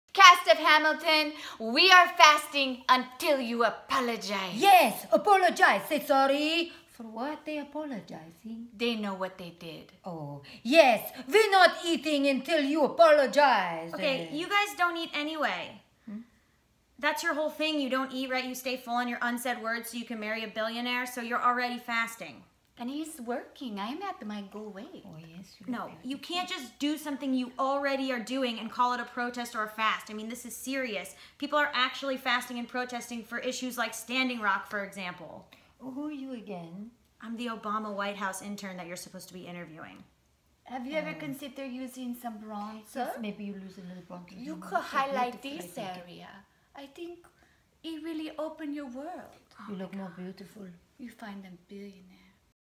Melania Trump's Cousins: Message to the Cast of Hamilton Musical with Gaby Dunn
The Cousins are fasting until they get an apology.